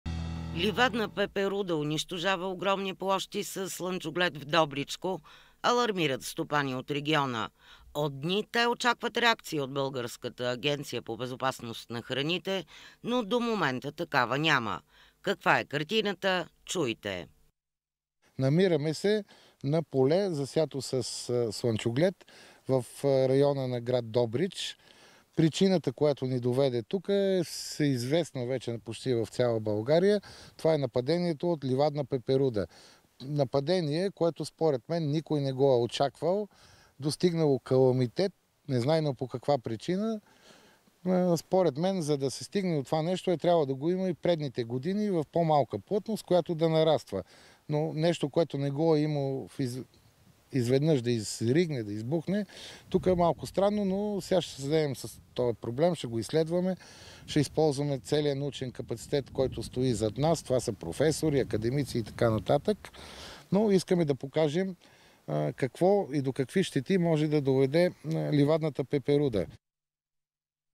Ливадна пеперуда унищожава огромни площи със слънчоглед
Ливадна пеперуда унищожава огромни площи със слънчоглед в Добричко, алармират стопани от региона. От дни те очакват реакция от Българската агенция по безопасност на храните, но до момента такава няма.